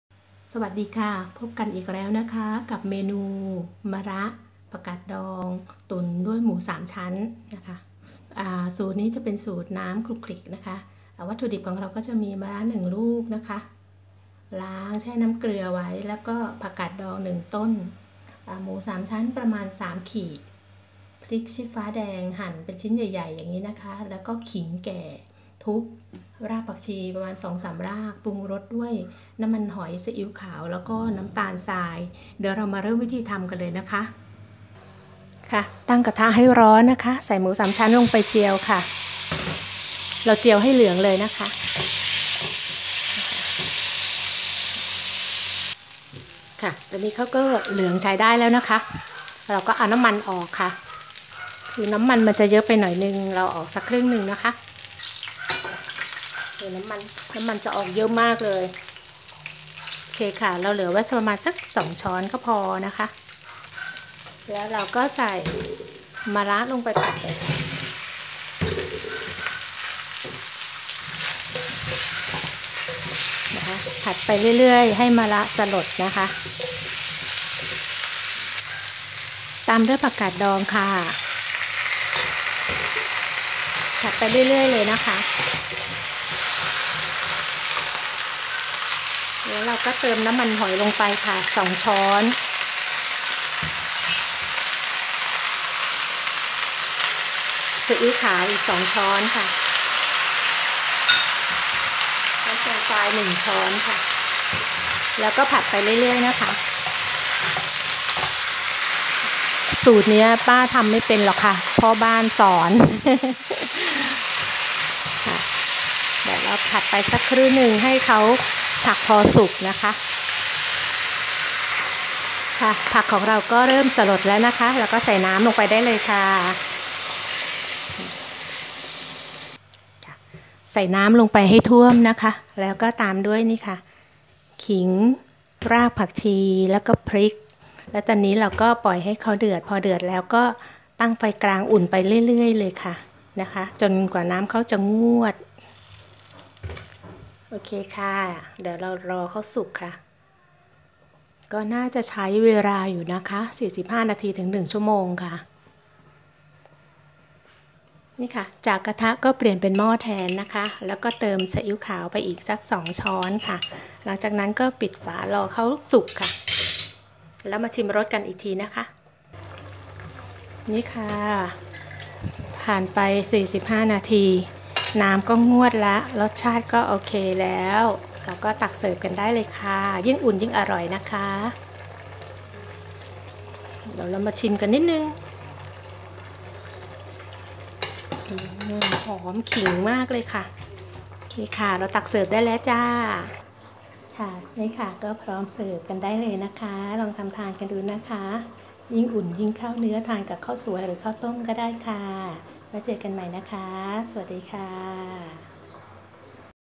ตุ๋นมะระผักกาดดองหมูสามชั้น หอมนุ่มยื่งอุ่นยิ่งอร่อย by คุณป้า ปอมปอม
ทานกับข้าวสวย หรือข้าวต้มอร่อยมากจ้า
ส่วนผสม มีดังนี้ค่ะ
1  มะระ 1ลูก ประมาณ 6 ขีด
2  ผักกาดดอง ครึ่งกิโลกรัม
3  ขิงแก่บุบพอแตก2แง่ง
4  พริกชีฟ้าหั่นหยาบๆ 3 เม็ด
5  รากผักชี 3 รากบุบ
6  หมูสามชั้น 4 ขีด
7 ปรุงรสด้วยน้ำตาลทราย1ช้อนโต๊ะ น้ำมันหอย 2 ชตซีอิ้วขาว 6 ชต (หรือปรุงตามชอบกันเลยนะคะ)

        เอาหมูสามชั้นลงทอดในกระทะไม่ต้องใส่น้ำมัน พอเหลืองน้ำมันก็จะออกมา(สูตรนี้ผัดน้ำมันเยอะๆอร่อยนะคะแต่ไม่ดีต่อสุขภาพเพราะป้าแก่แล้ว) ถ้าน้ำมันมากไปก็ตักออกบ้างก็ได้ค่ะ
        เอามะระ กับผักกาดดองลงไปผัด พอผ้ดผักสลดก็ปรุงรสตามชอบค่ะ แล้วใส่ขิง พริก รากผักชี
        เติมน้ำท่วมผัก ตุ๋นไฟกลางไปเรื่อยๆจนเปื่อยนุ่ม ชิมรสอีกครั้งค่ะ  ก็ทานได้เลยจ้า ถ้าทานไม่หมดแช่แข็งเก็บไว้ทานได้นานค่ะ
 ขอบคุณนะคะที่คิดตาม